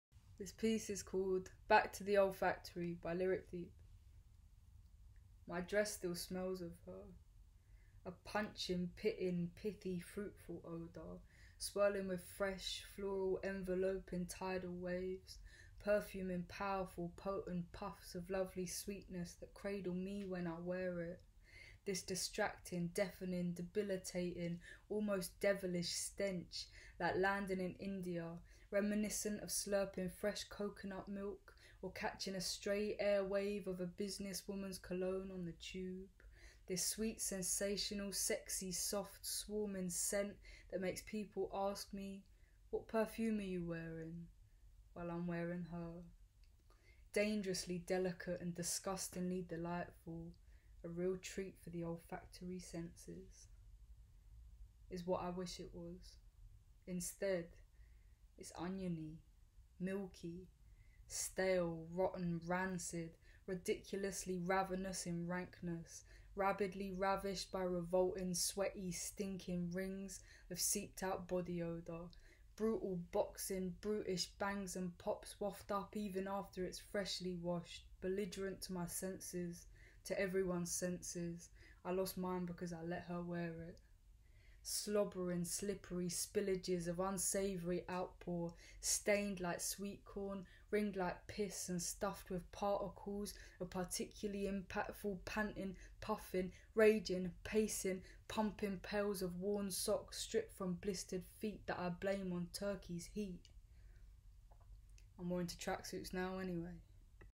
Back to The Olfactory by Lyric Deep
Back to The Olfactory

Words:

My dress still smells of her.

A punching, pitting, pithy, fruitful odour, swirling with fresh,

Floral, enveloping, tidal waves. Perfuming powerful, potent

Puffs of lovely sweetness that cradle me when I wear it.

This distracting, deafening, debilitating almost devilish stench

Like landing in India, reminiscent of slurping fresh coconut milk

Or catching a stray airwave of a businesswoman’s cologne

On the tube. This sweet, sensational, sexy, soft, swarming scent

That makes people ask me “What perfume are you Wearing?”

While I’m wearing her. Dangerously delicate and disgustingly delightful -

 A real treat for the olfactory senses.

Is what I wish it was.

Instead,

It’s oniony. Milky. Stale, rotten, rancid, ridiculously ravenous

In rankness. Rabidly ravished by revolting, sweaty, stinking rings

Of seeped out body odour. Brutal, boxing, brutish bangs and pops

Waft up even after it’s freshly washed. Belligerent to my senses.

To everyone’s senses. I lost mine because I let her wear it.

Slobbering, slippery spillages of unsavoury outpour, stained

Like sweet corn, ringed like piss and stuffed with particles

Of particularly impactful panting, puffing, raging, pacing

Pumping pales of worn sock stripped from blistered feet

That I blame on Turkey’s heat.

I’m more into tracksuits now,

Anyway

By Lyric Deep.

Written and performed by Lyric Deep. Copyrighted to Lyric Deep Ltd.